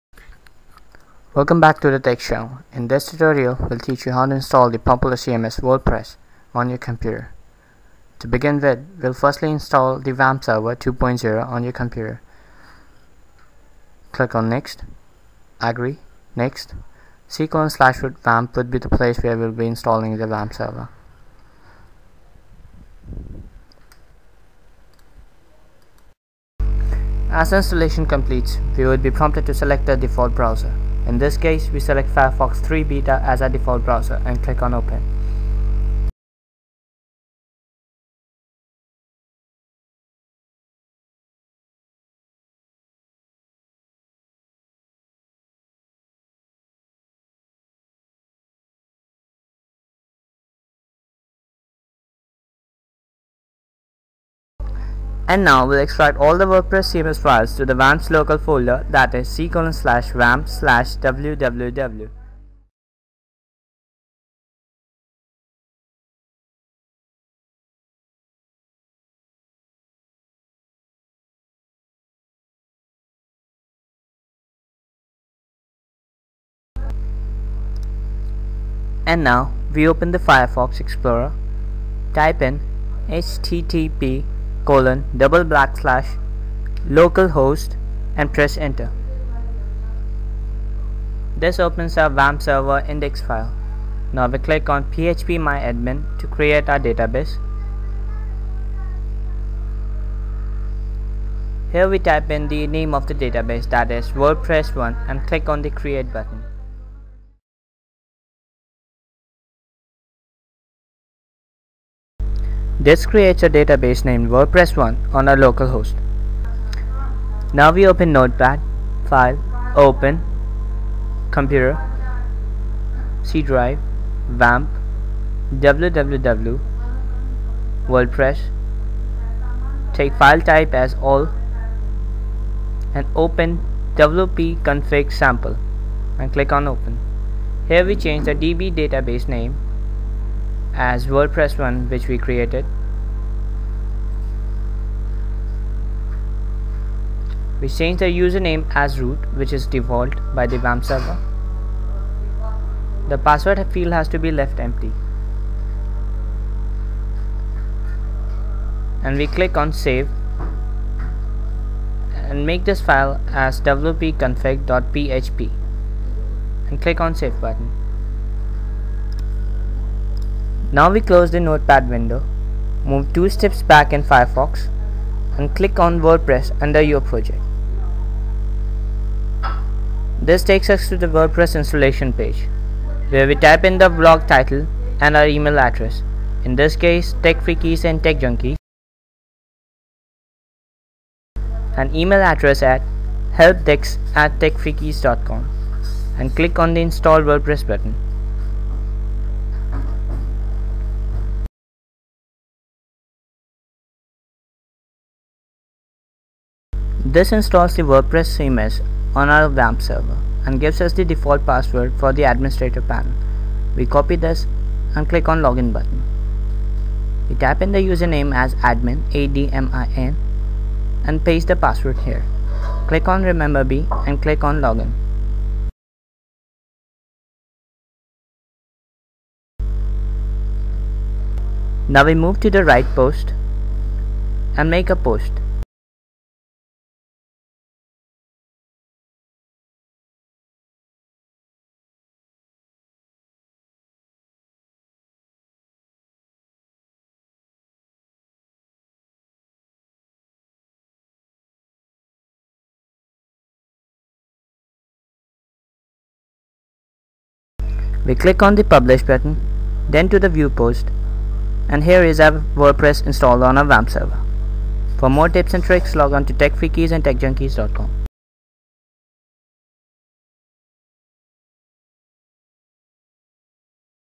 Install Wordpress on your Computer without Internet
This is a tutorial which explains how install the wordpress (popular CMS) on your computer without internet...